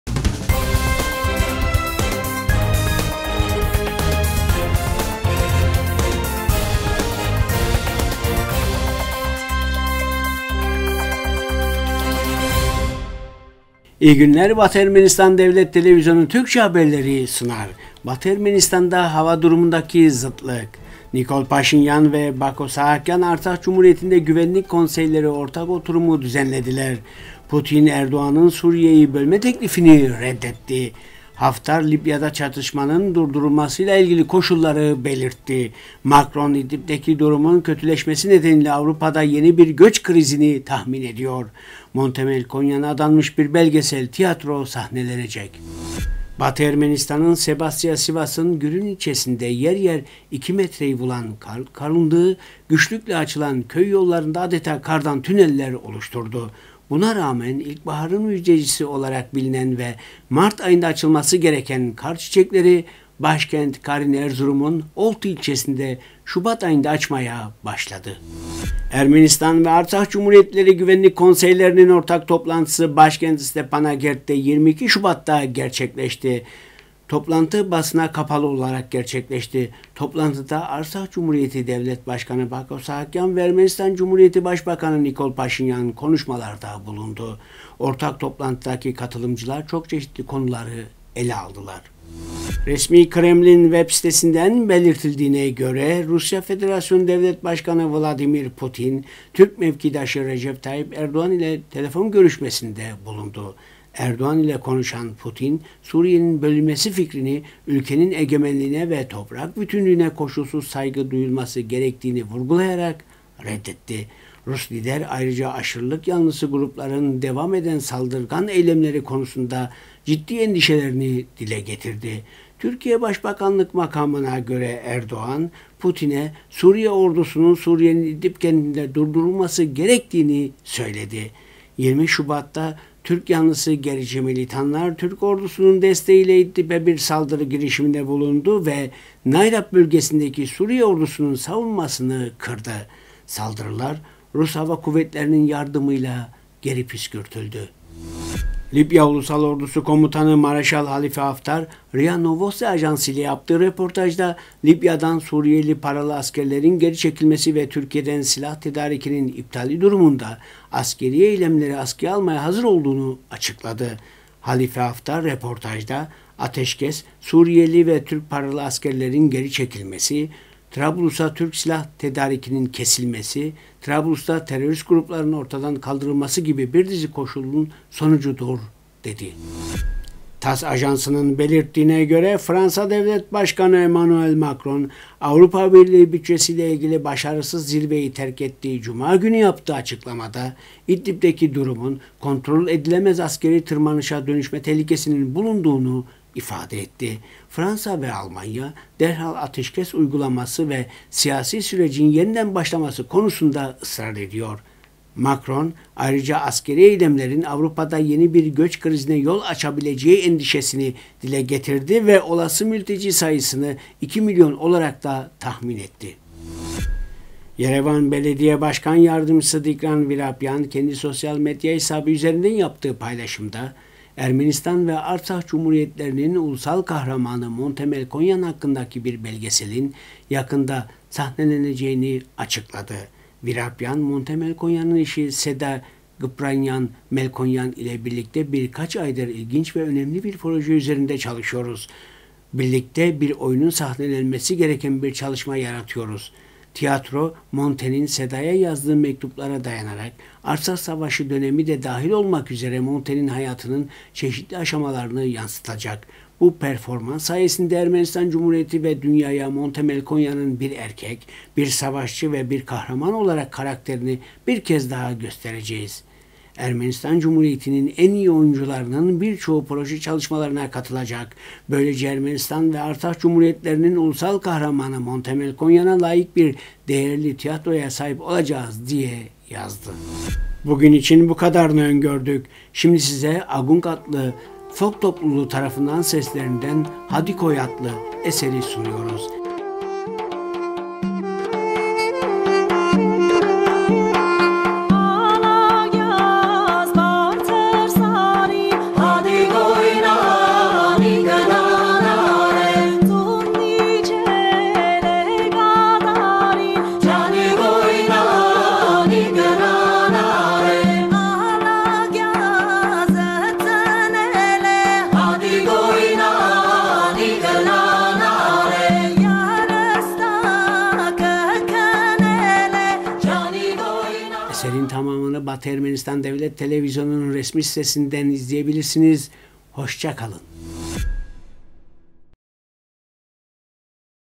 Batı Ermenistan’da hava durumundaki zıtlık.Batı Ermenistan haberleri 22-02-2020
1- Batı Ermenistan’da hava durumundaki zıtlık
2- Nikol Paşinyan ve Bako Sahakyan Artsakh Cumhuriyeti’nde Güvenlik Konseyleri ortak oturumu düzenlediler
3- Putin, Erdoğan'ın Suriye'yi bölme teklifini reddetti
4- Haftar Libya'da çatışmanınn durdurmasıyla ilgili koşulları belirtti
5- Macron, İdlib'deki durumun kötüleşmesi nedeniyle Avrupa'da yeni bir göç krizini tahmin ediyor
6- Monte Melkonyan'a adanmış bir belgesel tiyatro sahnelenecek